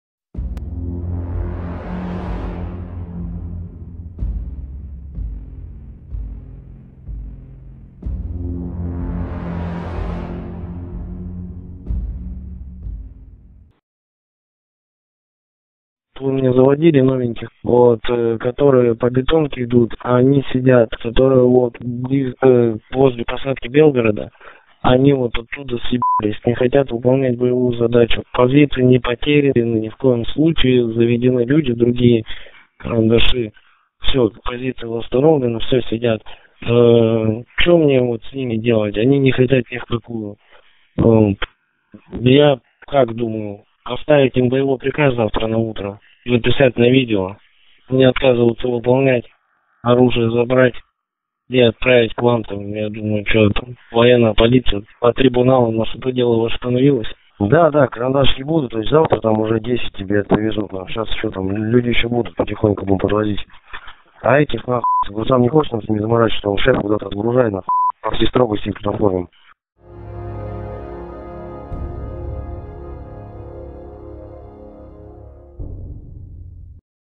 "Шли их на*уй, если не хочешь заморачиваться — окупанти про своїх військових | Перехоплення ГУР
Окупанти спілкуються між собою. Один з них доповідає про невиконання підлеглими бойової задачі та озвучує своє бачення їх покарання.

Побач більше!